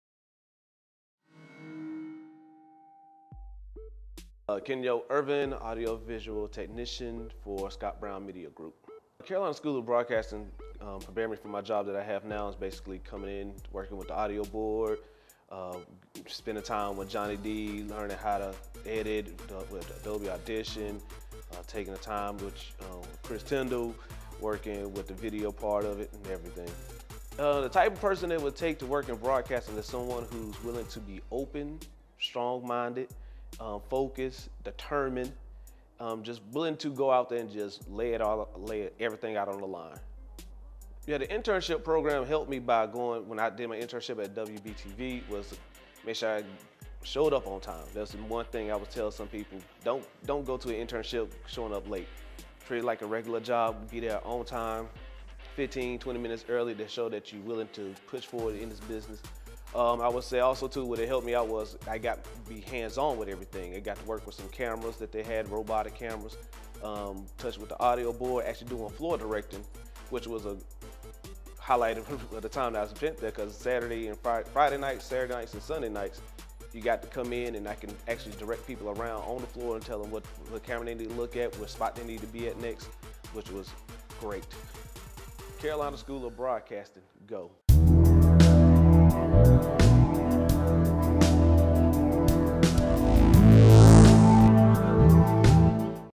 Kenyo Ervin Testimonial
Hear Kenyo Ervin talk about his experience here at the Carolina School of Broadcasting and how it helped prepare him for his current job as an AV Tech.